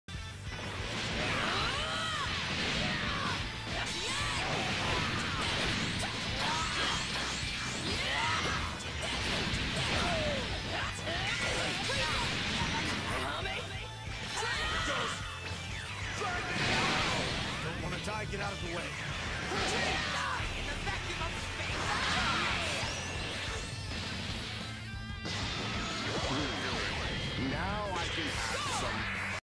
TGohan + Tien + Frieza 214L Round Start ToD
0 bar start (Sparking Lv1)
214L Sparking 2S 5M JM(Tien Assist)H 2S 66 2S 236LMMMM 44 2S 2S JLMH J236L Hold (Frieza Assist*) 214L AA Kamehameha Tien DHC 66 Tribeam Frieza DHC

* Call Frieza Assist as Gohan is landing from J236L

Tien Lv1 super damage is always silly